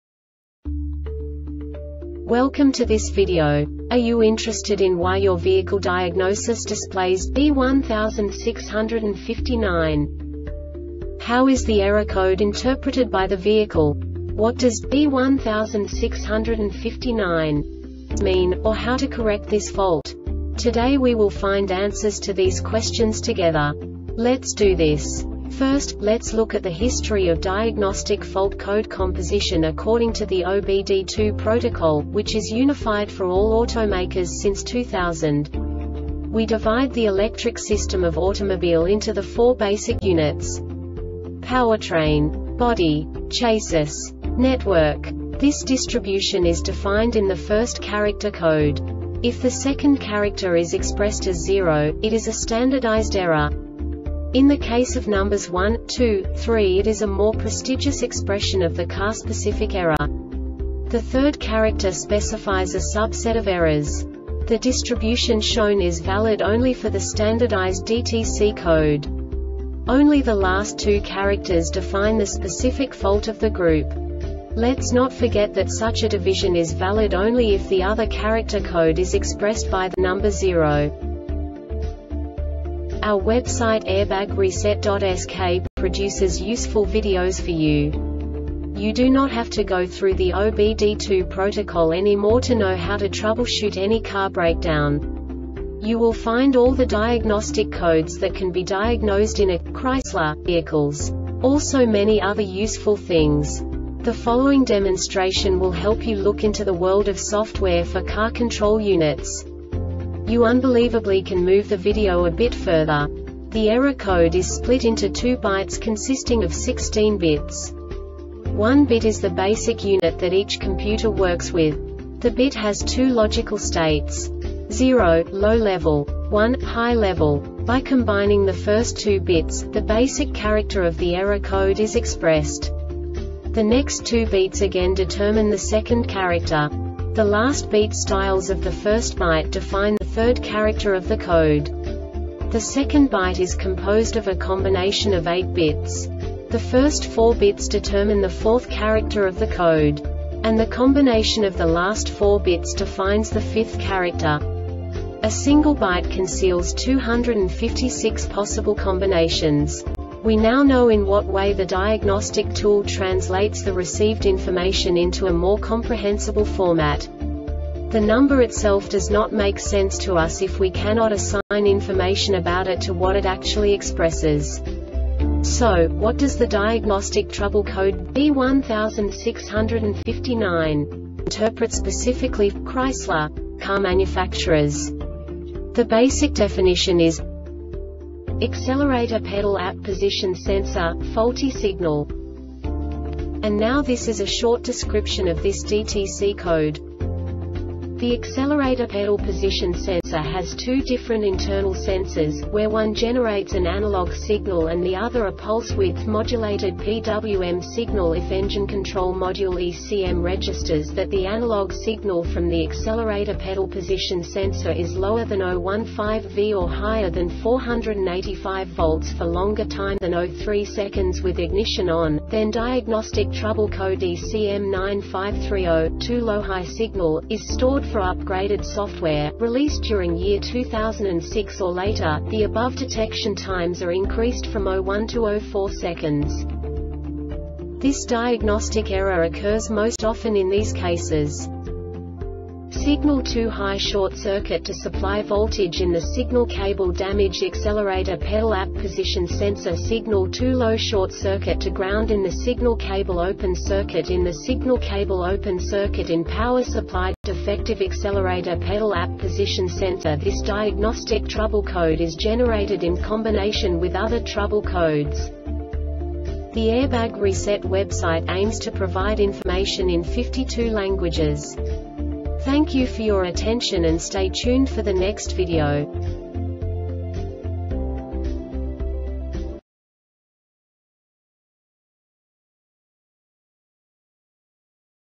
DTC Chrysler B1659 Short Explanation
Contents:
0:21 Basic DTC analysis according to OBD2 protocol standard.
1:48 Insight into programming
2:58 A diagnostically understandable description of the Diagnostic Trouble Code
3:05 Basic error definition

Make:
Chrysler
Code:
B1659
Definition:
Accelerator Pedal (AP) Position Sensor, Faulty Signal
Description:
The accelerator pedal position sensor has two different internal sensors,where one generates an analogue signal and the other a pulse-width modulated (PWM) signal.If Engine control module (ECM) registers that the analogue signal from the accelerator pedal position sensor is lower than 0.15 V or higher than 4.85 V for longer time than 0.3 seconds with ignition on,then diagnostic trouble code ECM-9530, Too low or high signal,is stored.For upgraded software,released during year 2006 or later,the above detection times are increased from 0.1 to 0.4 seconds.
Cause:
Signal too high:Short-circuit to supply voltage in the signal cableDamaged accelerator pedal (AP) position sensor.Signal too low:Short-circuit to ground in the signal cableOpen-circuit in the signal cableOpen-circuit in power supplyDefective accelerator pedal (AP) position sensorThis diagnostic trouble code is generated in combination with other trouble codes